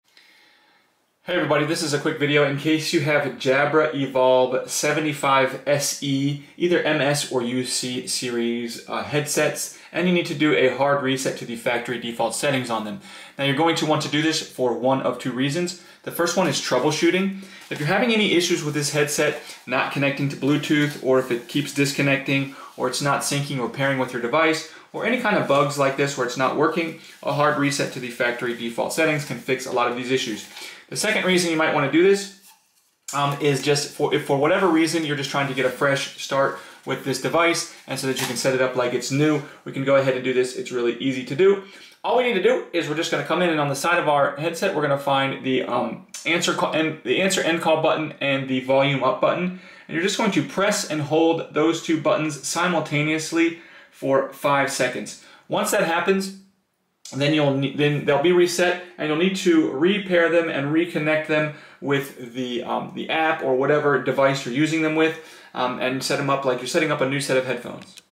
Hard Reset Jabra Evolve 75 SE MS UC Headset
How to troubleshoot or setup starting with a force hard reset a Jabra Evolve 75 SE MS/UC Headset wireless bluetooth Headphones/earphones to factory default settings if it is not working or not connecting or if it is frozen or not responding, the factory reset is a good troubleshooting step. You can also restore the factory settings to setup your device as new. Found in the Jabra Evolve 75 SE MS/UC Headset user manual.